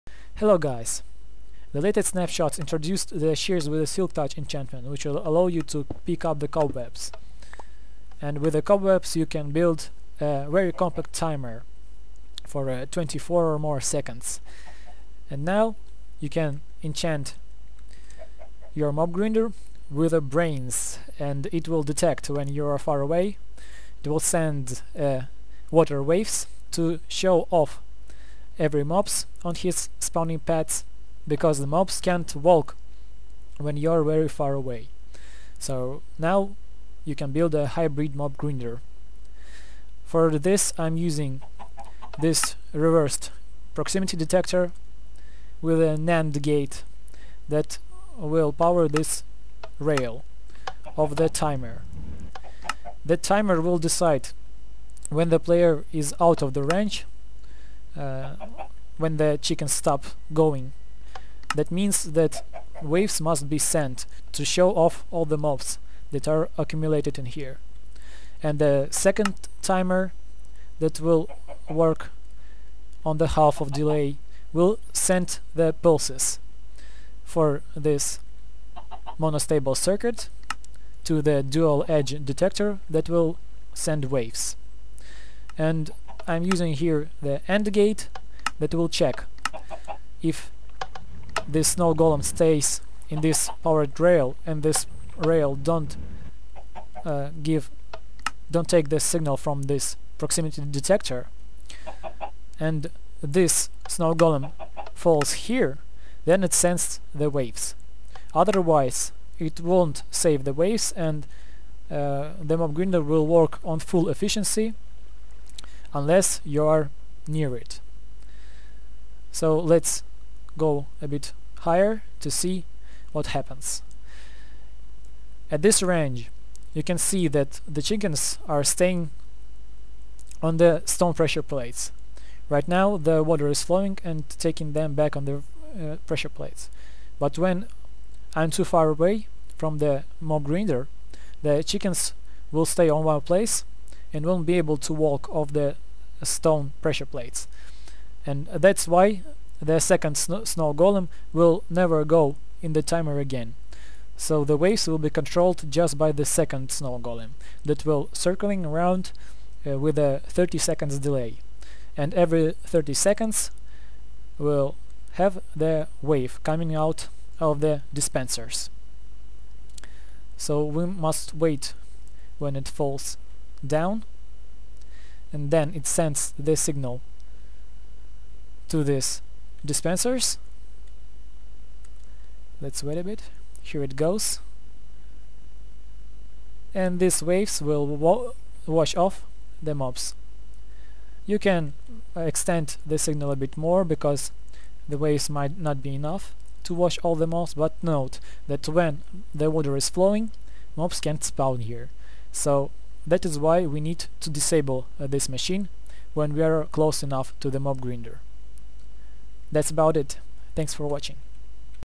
Smart Mob Grinder (snapshot 12w50b)
This system will change your standard mob grinder to a washer type, when you are far away and mobs can't leave spawning pads by themselves.
The reversed Proximity Sensor resets the first (1 min) timer when player is near the mob system. The second timer will generate pulses when first timer is idle. The pulses will travel through Monostable Circuit and Dual Edge Detector to generate waves.

I didn't implement it in real mob grinder because of the broken Mob AI in the snapshot. I'll put it in UMG later.